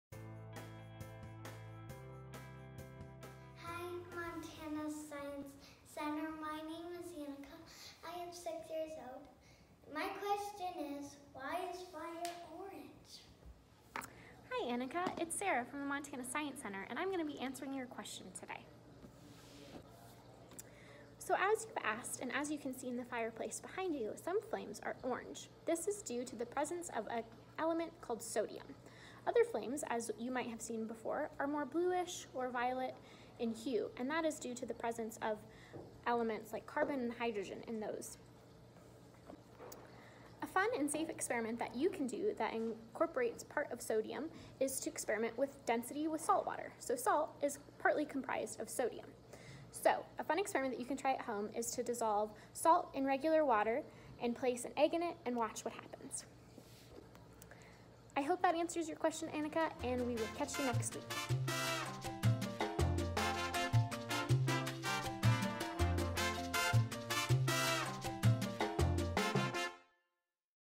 MSC How Come Series Ep 5: Fire
In this How Come episode, Annika asks why fire is orange. Try out your own experiment!